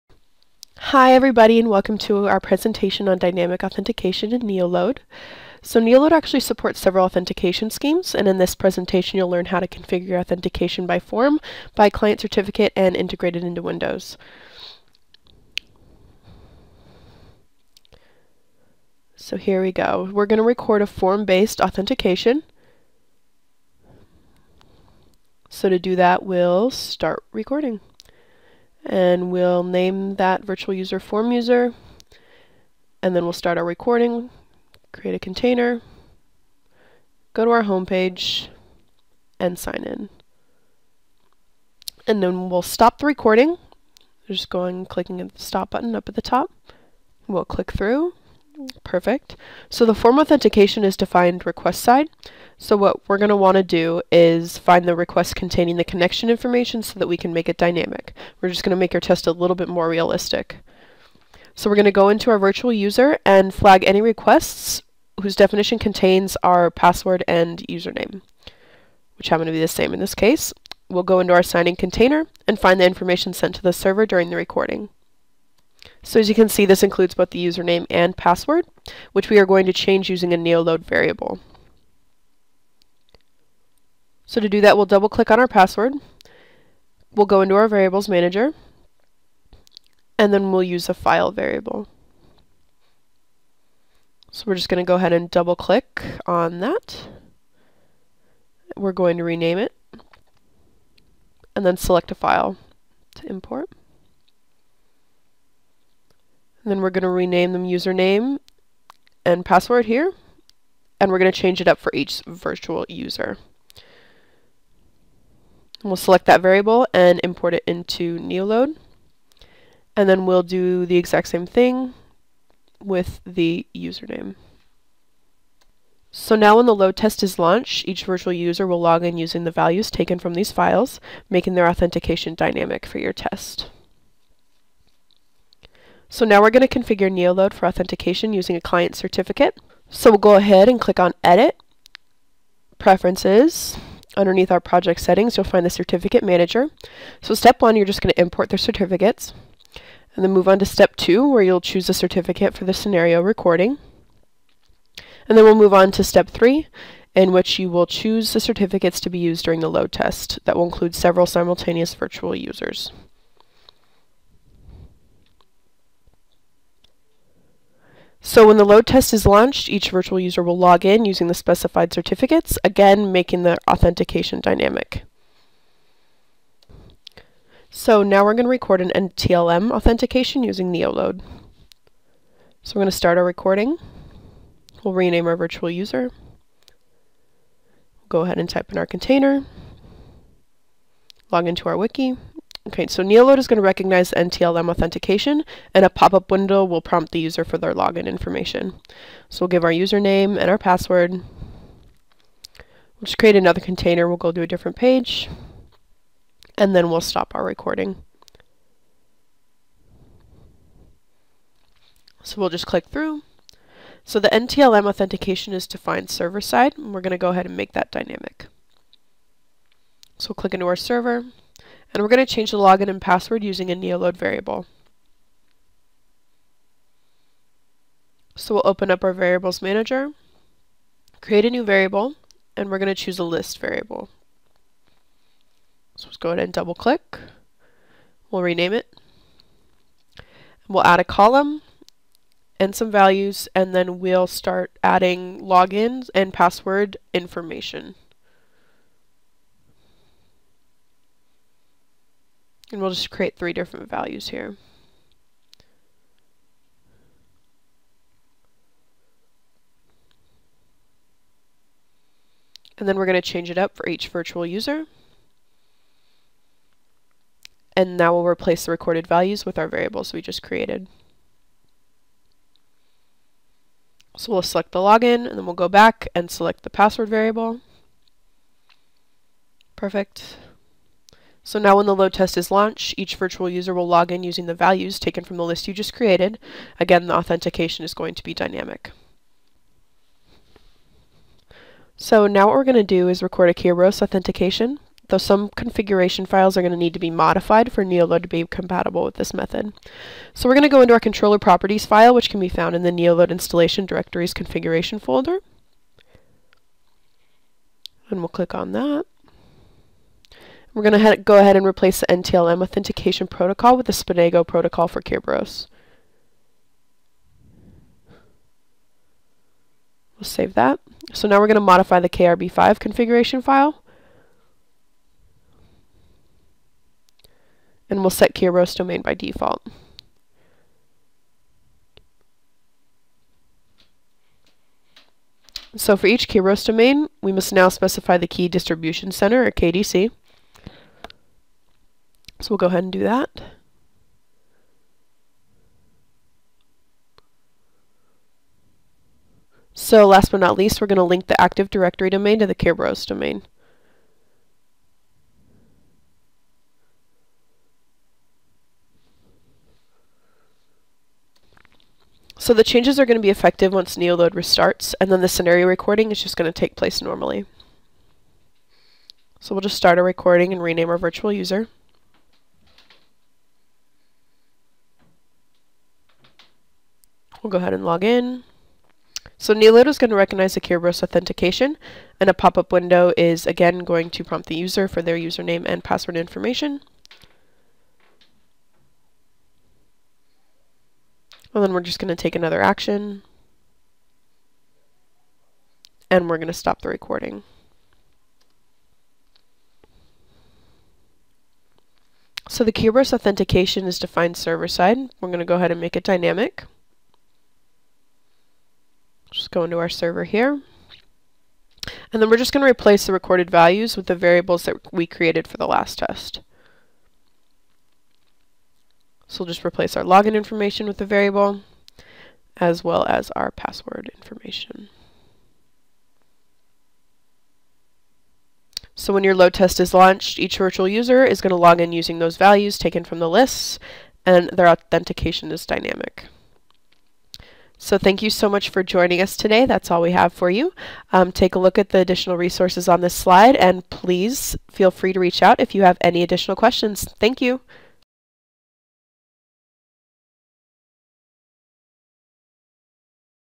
Stress Testing Dynamic Login with NeoLoad
NeoLoad is a Web Testing Tool ensuring the performance and robustness of your web application realistic load. Click to see how easily you can test your application in 10 minutes.